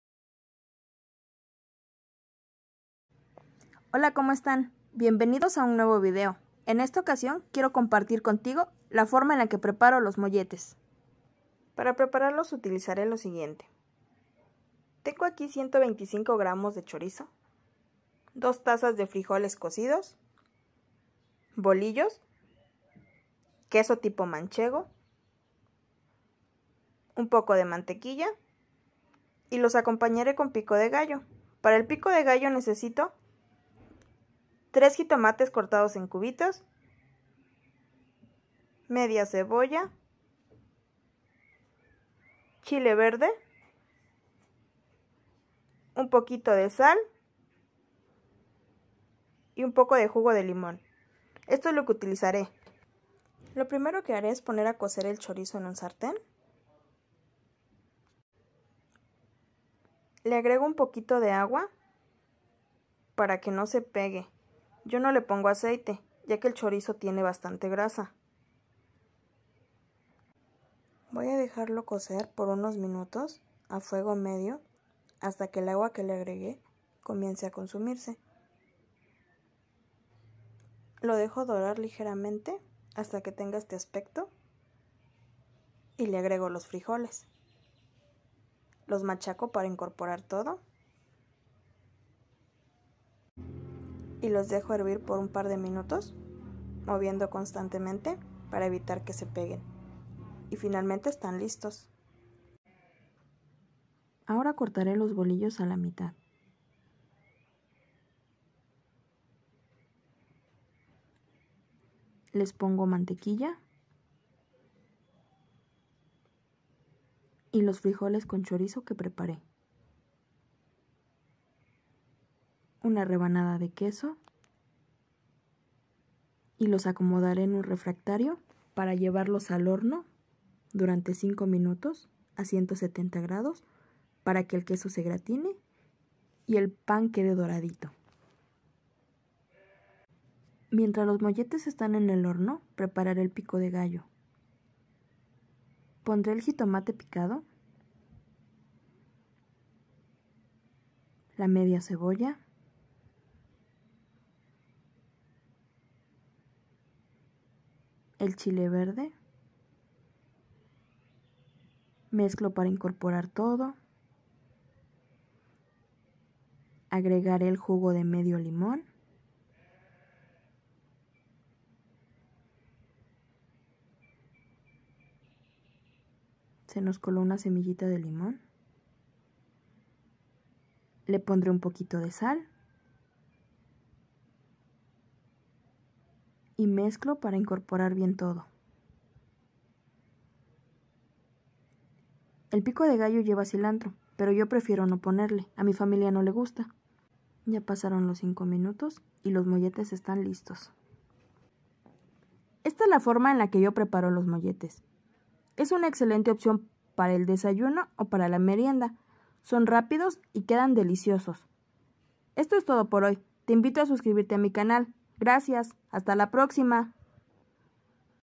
Molletes fácil y rápido 👌😋😋
la forma en la que preparó unos deliciosos molletes para el desayuno o la merienda